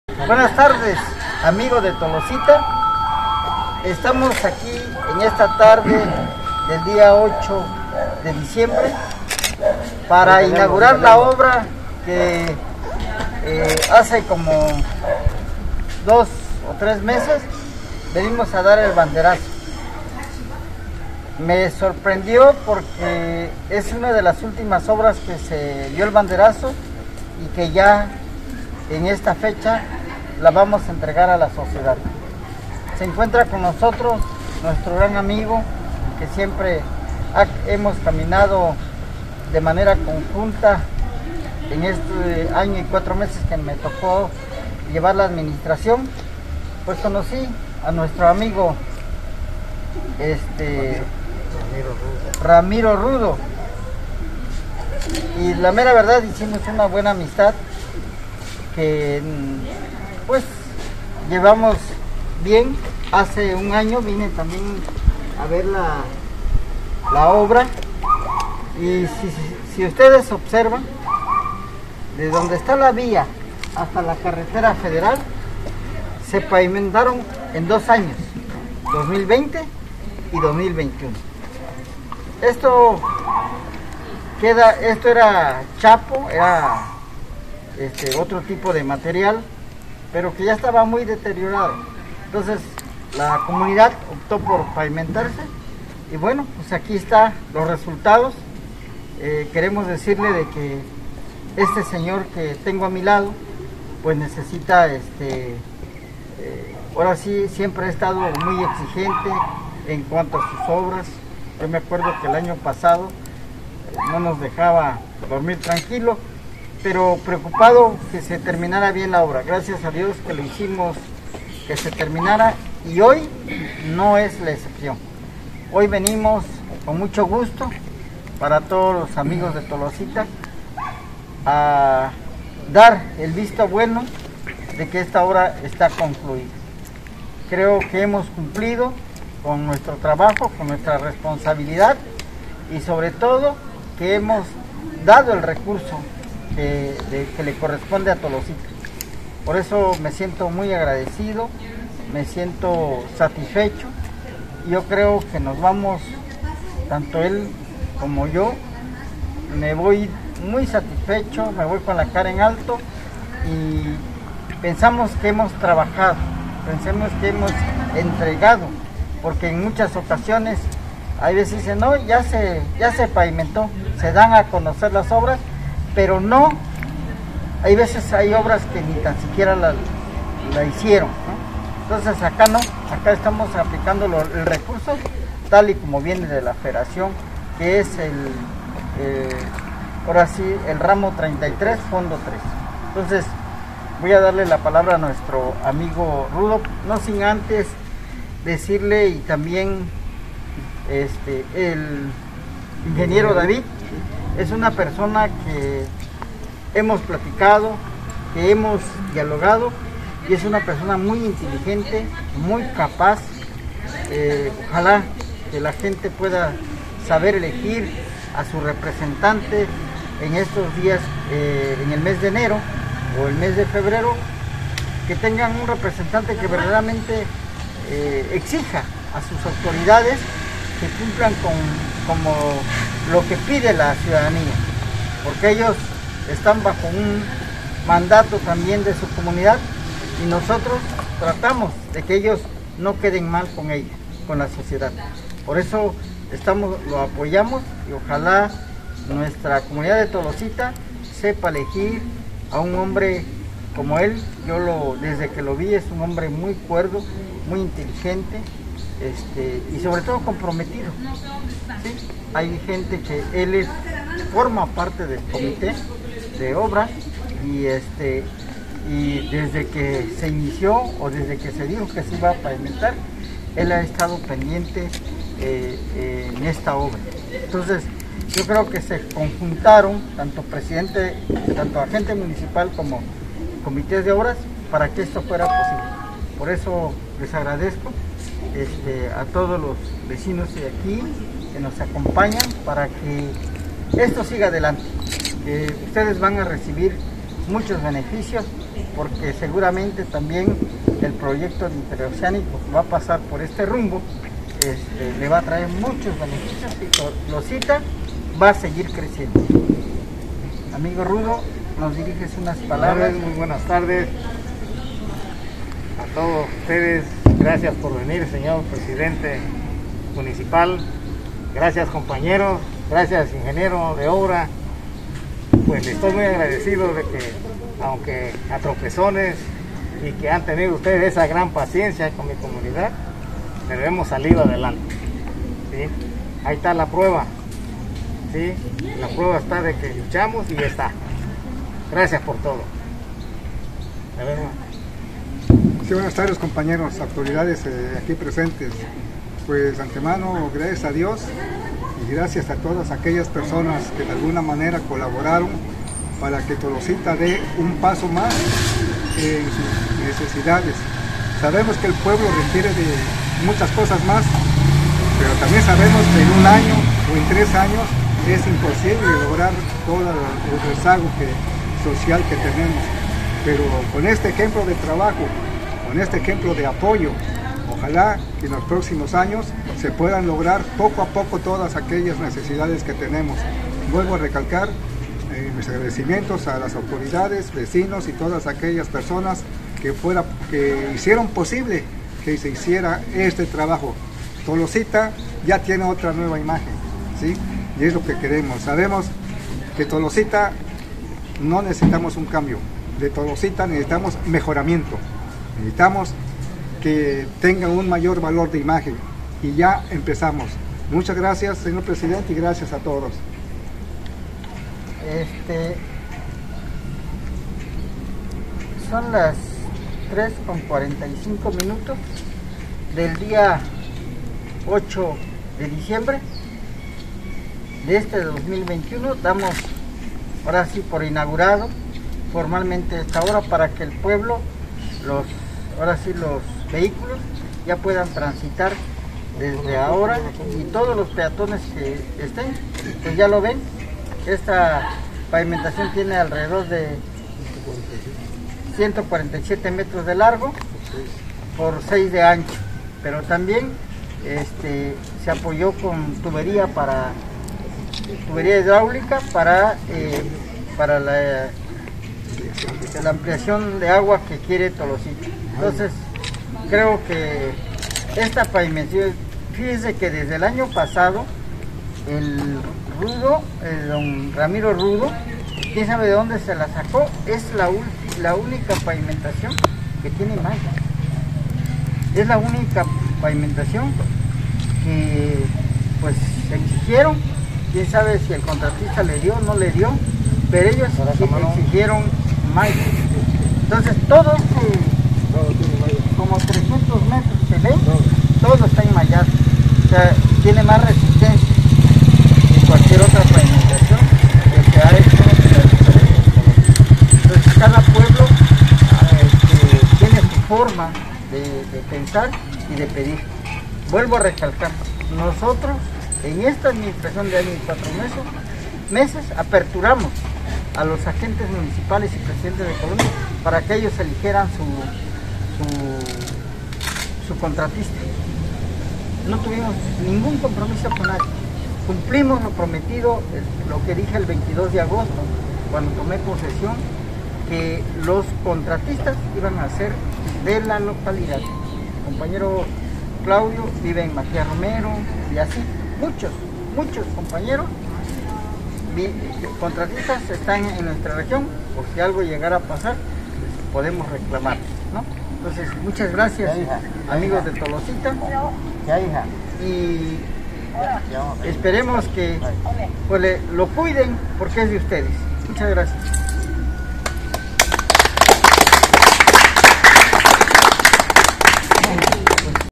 INAUGURACIÓN DE PAVIMENTACIÓN DE LA CALLE EMILIANO ZAPATA
EL PRESIDENTE MUNICIPAL CONSTITUCIONAL DEL MUNICIPIO DE MATIAS ROMERO AVENDAÑO, C. MANUEL SOLANA MORALES INAUGURO LA SEGUNDA ETAPA DE PAVIMENTACIÓN DE LA CALLE EMILIANO ZAPATA EN LA POBLACIÓN DE TOLOSITA, OAXACA, ESTANDO PRESENTES EL CONTRATISTA, COMITÉ DE OBRAS Y PERSONAS DE LA COMUNIDAD.